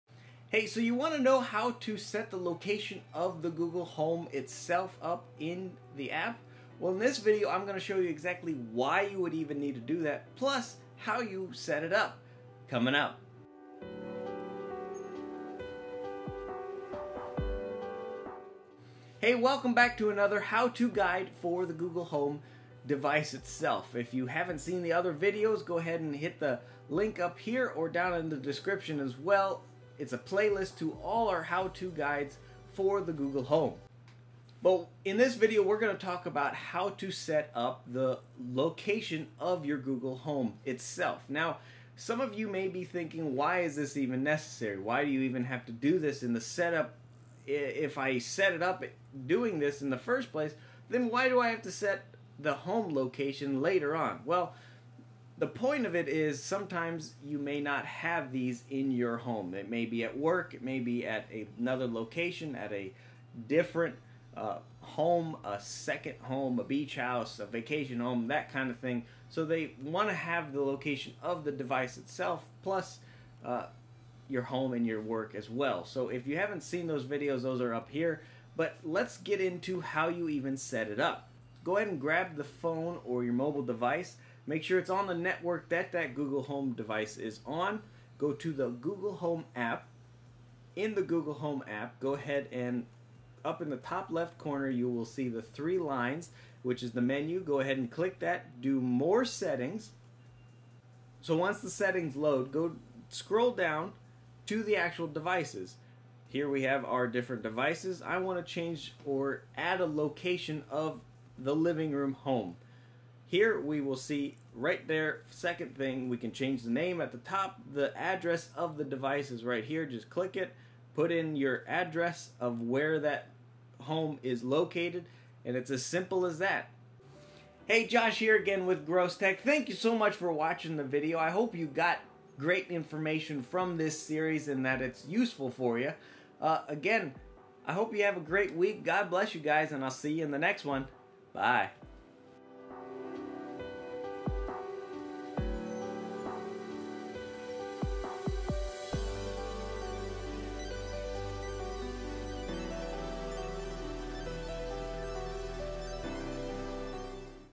How to set the device location on the google home
There are so many different settings in the Google Home how do you know what you have to and why? Well in this video I'm going to show you how to set the device location on the Google Home and why it's even something that you have to do.
►BACKGROUND MUSIC
→Epic Music

In this video I take you through how you can setup your location of the Google Home itself, and why its even necessary to do something like that.